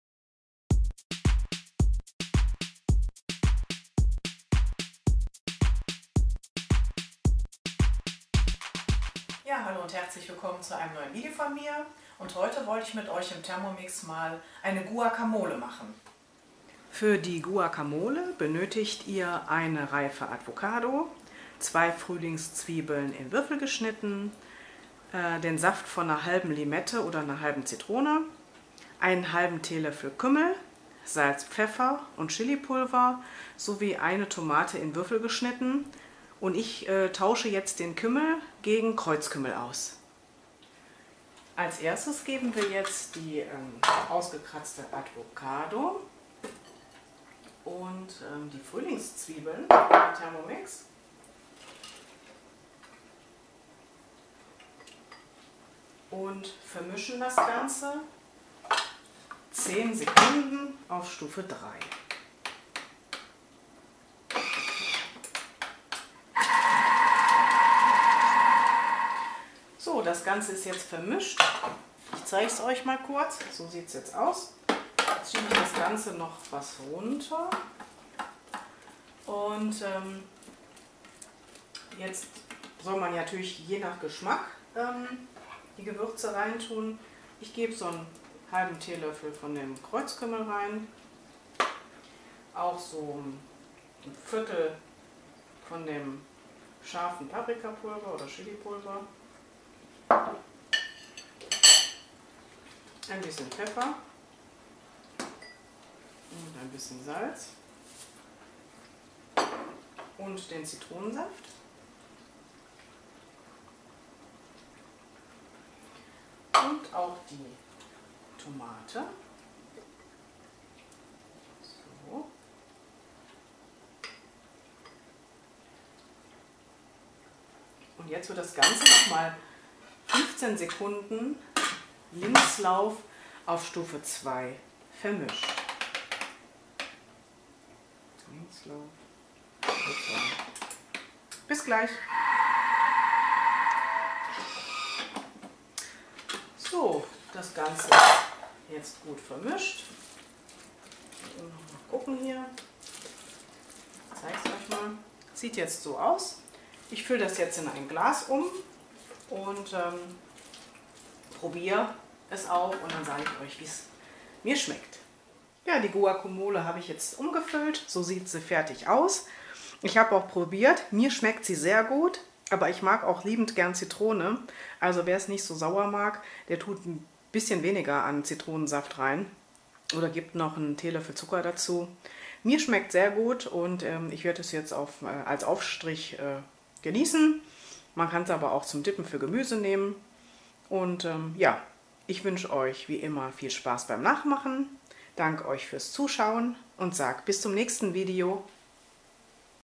Guacamole - Thermomix®TM5®
Diese Guacamole ist super lecker und im Thermomix ganz fix zubereitet.